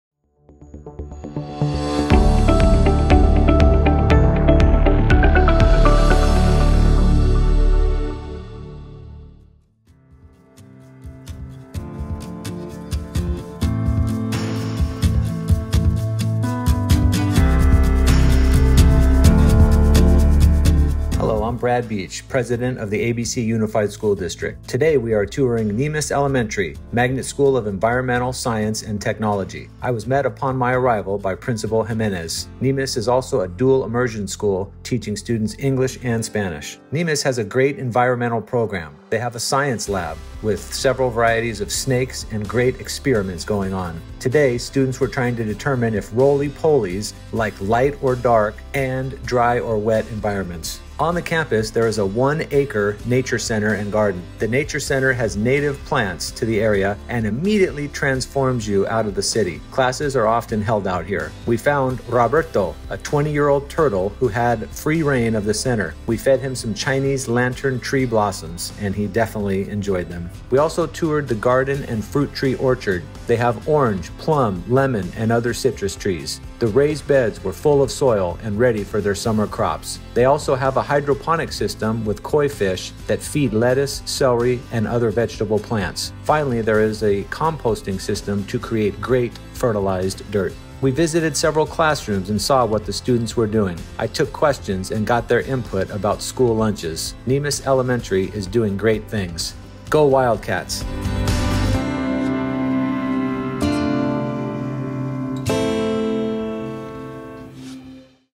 30 in 30 - Niemes Elementary
School Board President Brad Beach visits  30 schools in 30 weeks to get an on the ground view of the students and staff at every ABC campus.